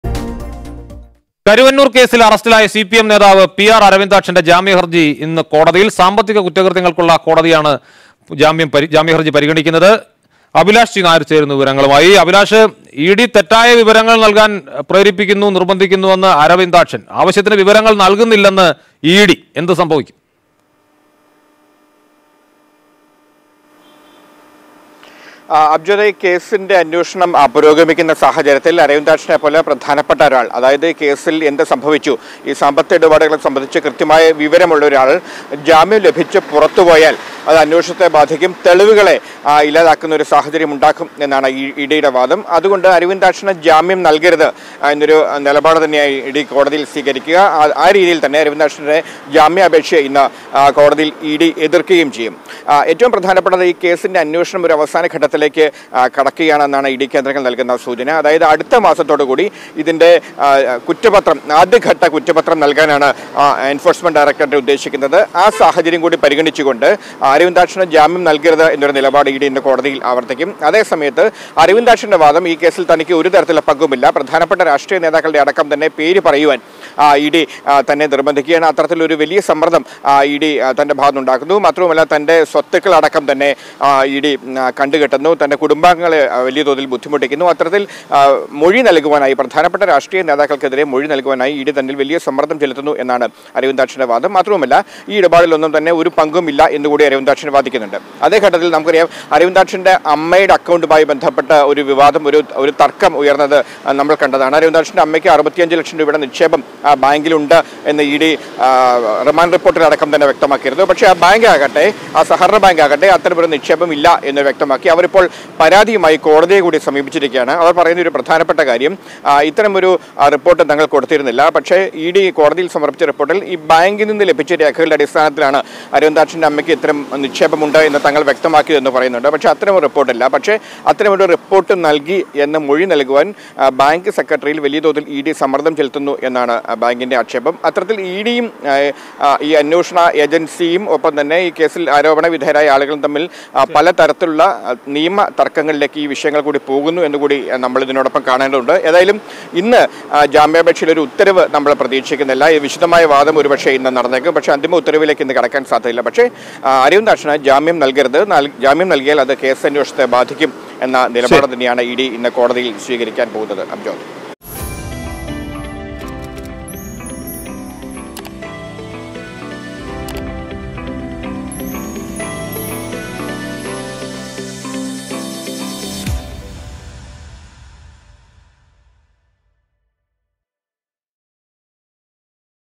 പി.ആർ.അരവിന്ദാക്ഷന്റെ ജാമ്യഹർജി ഇന്ന് കോടതിയിൽ | Karuvannur Bank Scam | PR Aravindakshan
കരുവന്നൂർ കേസിൽ അറസ്റ്റിലായ സിപിഎം നേതാവ് പി.ആർ.അരവിന്ദാക്ഷന്റെ ജാമ്യഹർജി ഇന്ന് കോടതിയിൽ; സാമ്പത്തിക കുറ്റകൃത്യങ്ങൾക്കുള്ള കോടതിയാണ് ഹർജി പരി​ഗണിക്കുക, തെറ്റായ വിവരങ്ങൾ നൽകാൻ ഇഡി സമ്മർദ്ദമെന്ന് അരവിന്ദാക്ഷൻ
Karuvannur Bank Scam | ED | Kochi | PR Aravindakshan  | CPM